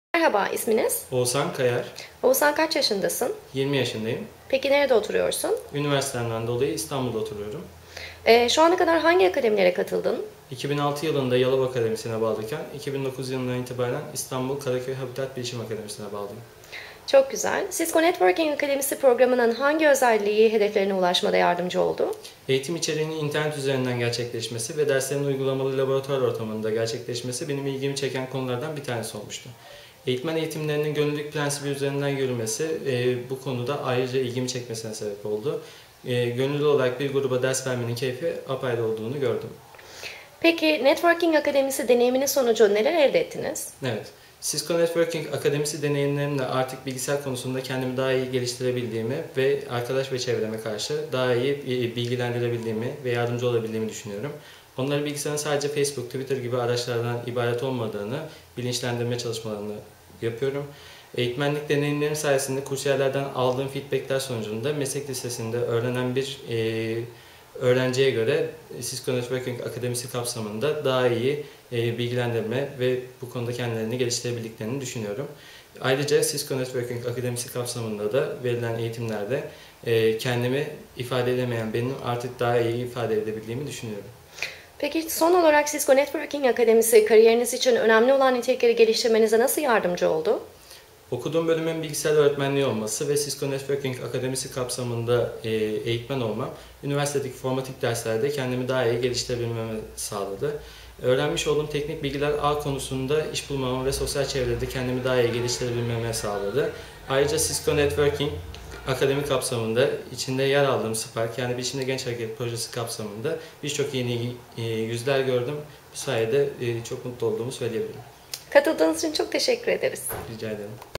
Oguzhan Kayar - Cisco Networking Academy Success Story
Oguzhan Kayar, one of the Master Instructors of SPARK and Spark for Women projects in Turkey, is sharing his views on what its like to be a part of Cisco Networking Academy program.